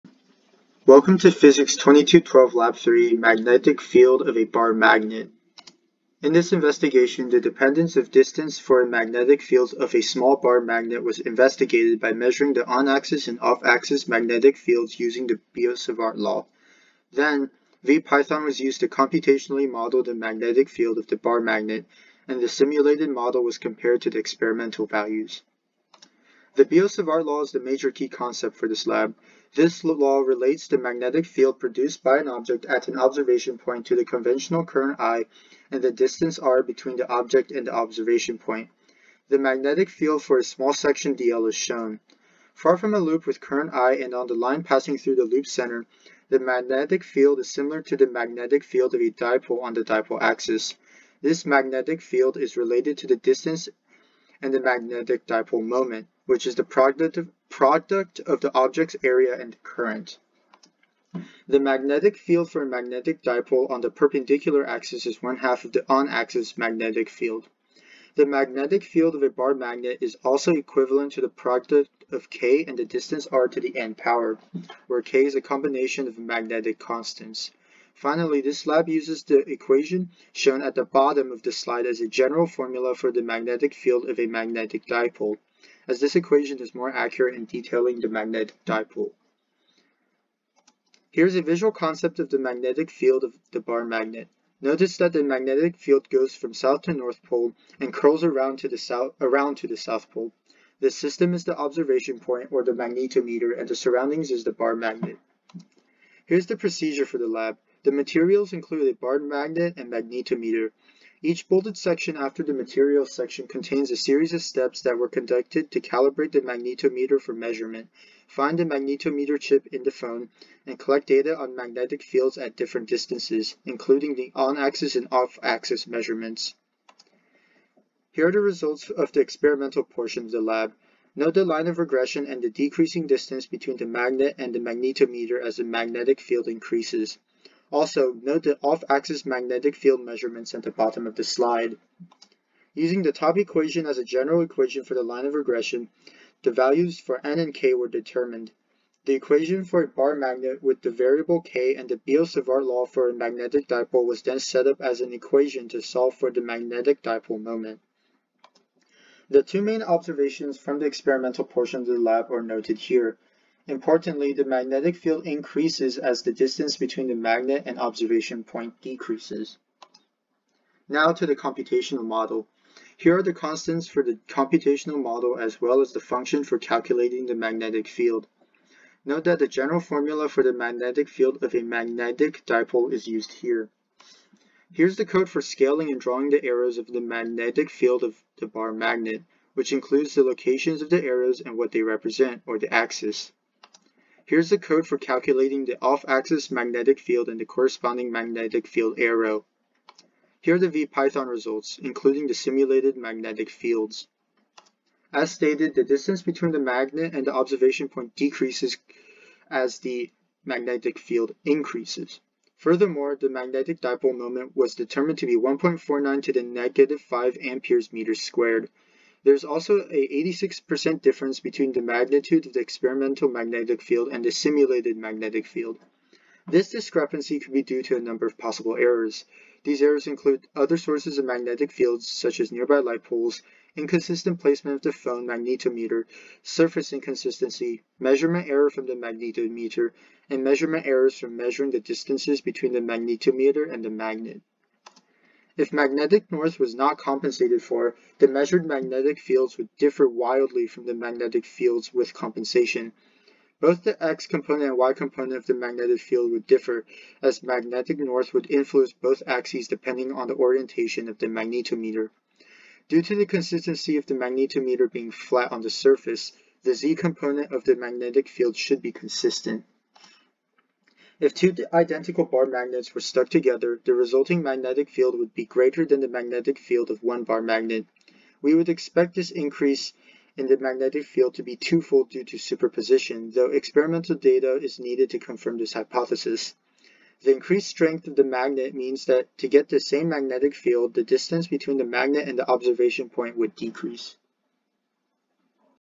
PHYS 2212 Lab 3 - Magnetic Field of a Bar Magnet
Lab 3 of PHYS 2212, the magnetic field of a bar magnet problem. From William Yang of the Georgia Institute of Technology, Fall 2022.

This lab investigates the magnetic field of a bar magnet due to changes in the distance between the magnet and some observation point (magnetometer). Using the Biot-Savart Law and relevant magnetic field equations, an experiment was conducted to determine the relationship between the magnetic field and the distance between the observation point and the magnet. Then, VPython was used to code a theoretical magnetic field based on the values found in the experiment. The simulated magnetic field was compared to the experimental results.